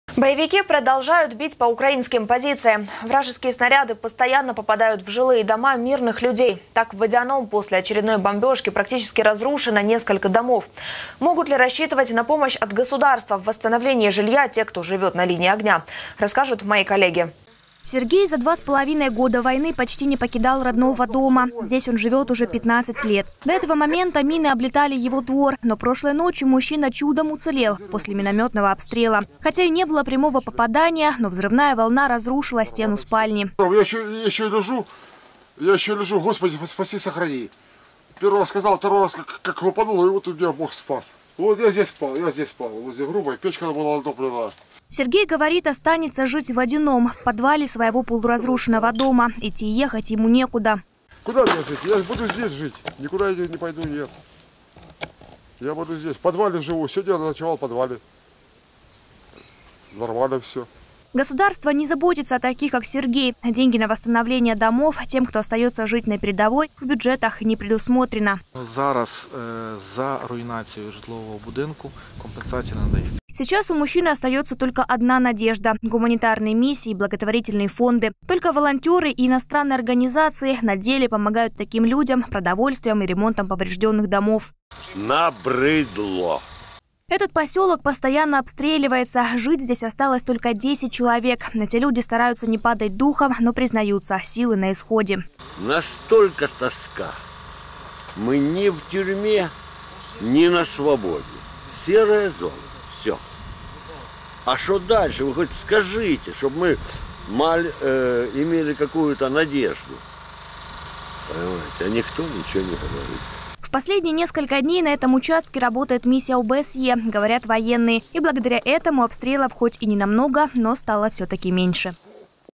В Водяном Донецкой области снаряды снова разрушили дома мирных жителей
Боевики продолжают бить по украинским позициям. Вражеские снаряды постоянно попадают в жилые дома мирных людей. Так в Водяном после очередной бомбежки практически разрушено несколько домов. Могут ли рассчитывать на помощь от государства в восстановлении жилья те, кто живет на линии огня? Узнайте в сюжете.

 Мы рядом!